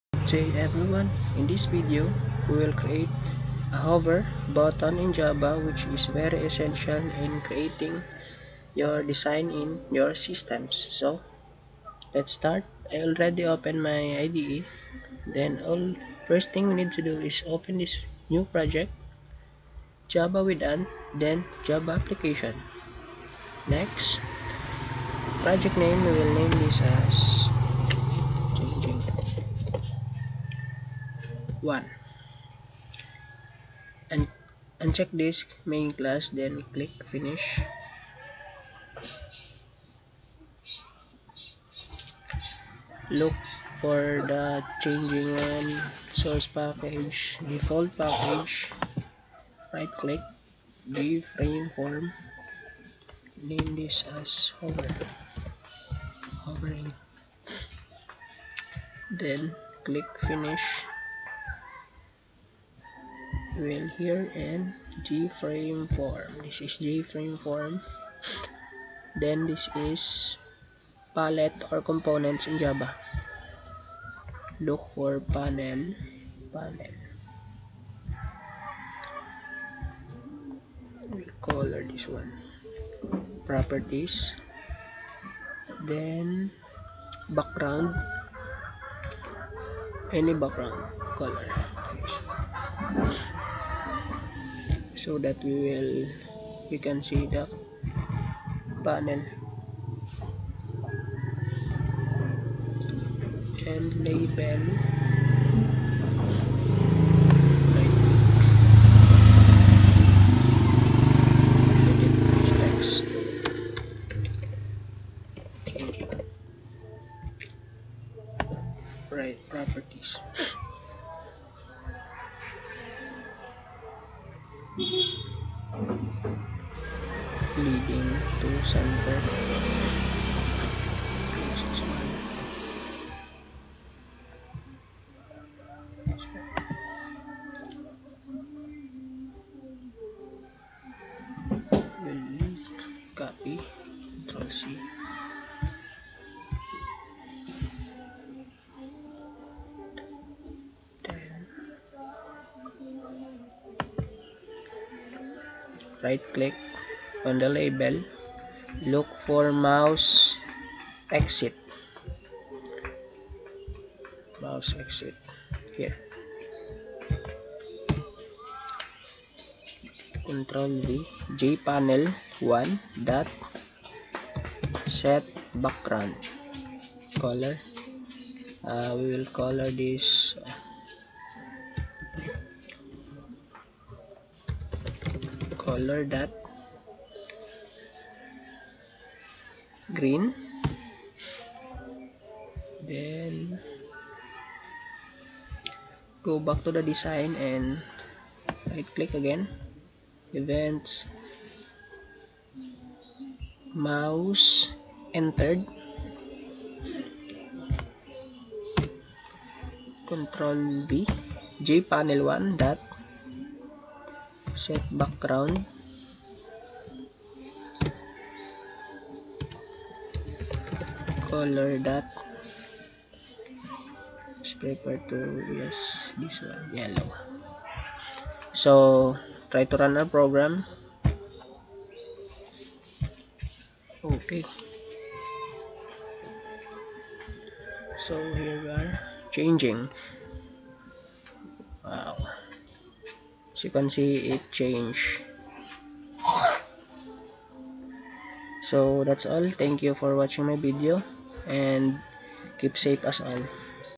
How to create a hovering button in java or changing the color of the buttin in java.
freesourcecode This is essential in designing buttons in java free source code